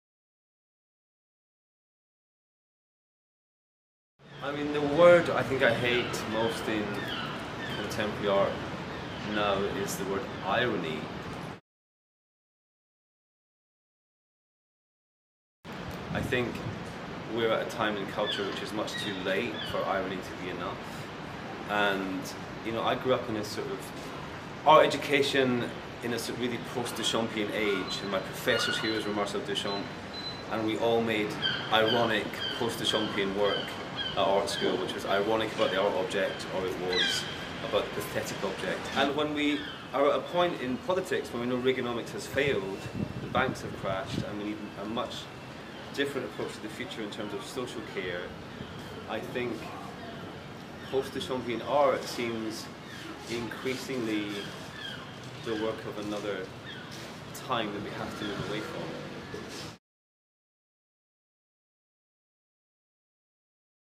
Jargon: Robert Montgomery on Irony
Scottish artist, Robert Montgomery discusses the artspeak term that most infuriates him.